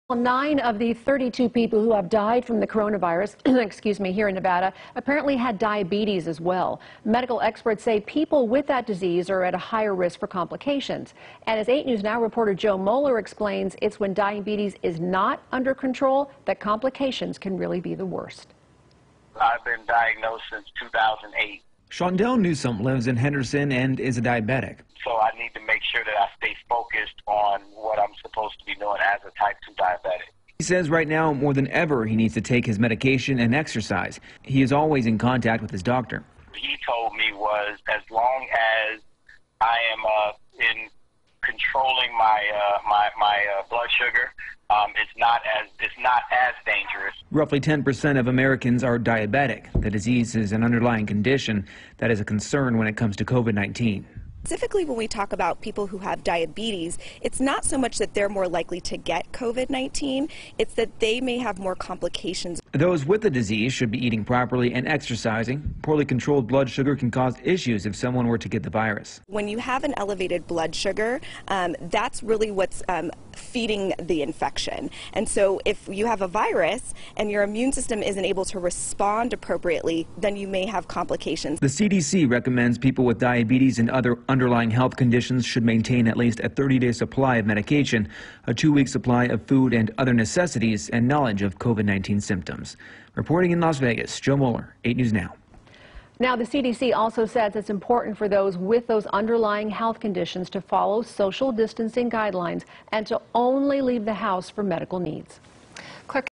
People with diabetes at higher risk for coronavirus complications
Nine of the 32 people who have died from the coronavirus in Nevada had diabetes as well.  Medical experts say people with the disease are at a higher risk of complications.